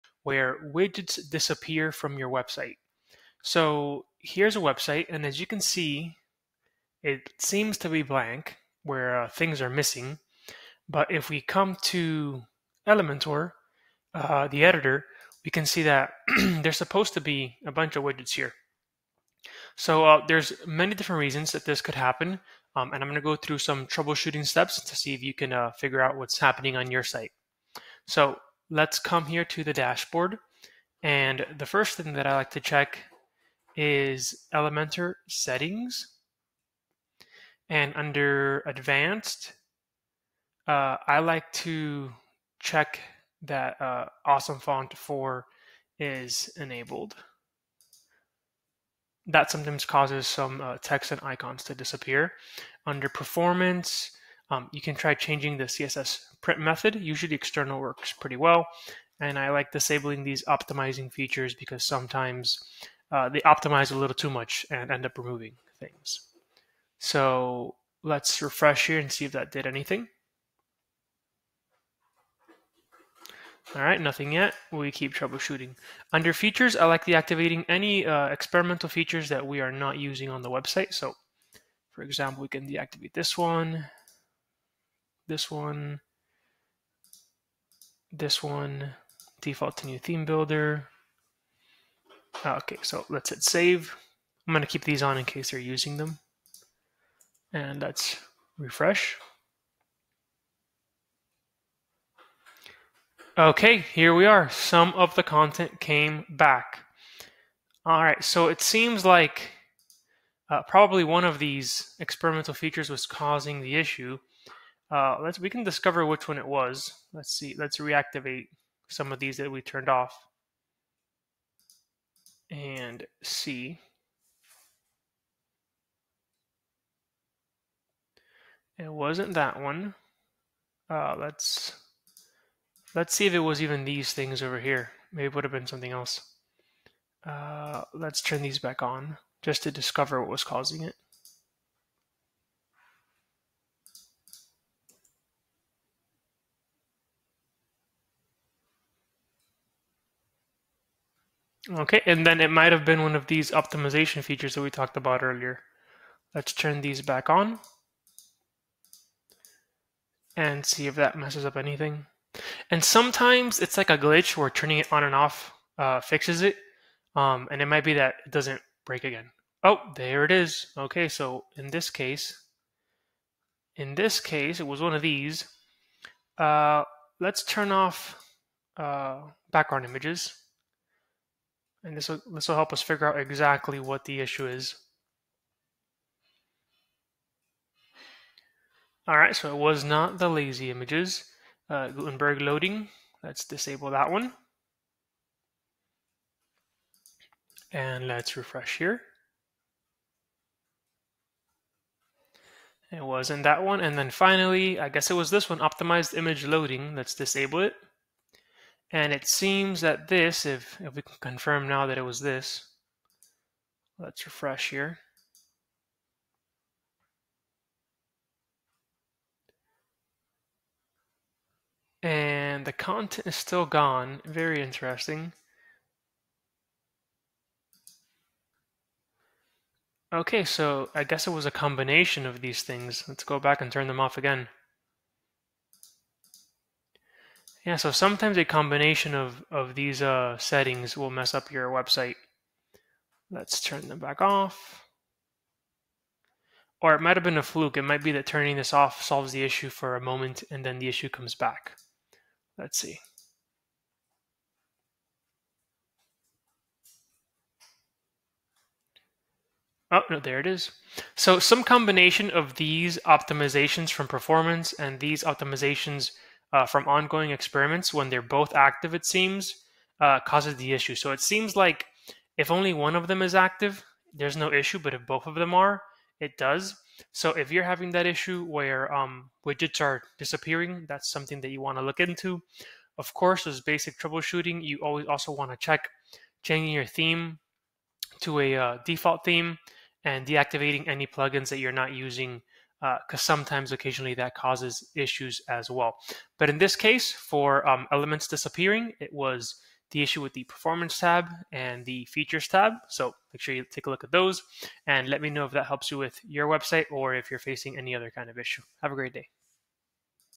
How To Fix Missing Elementor Elements On Live Site
If you are using elementor and notice the elements are loading correctly on the editor but missing on the live site, here are some troubleshooting steps you can take to correct the issue! I hope this helps! Let me know if you have any other elementor questions!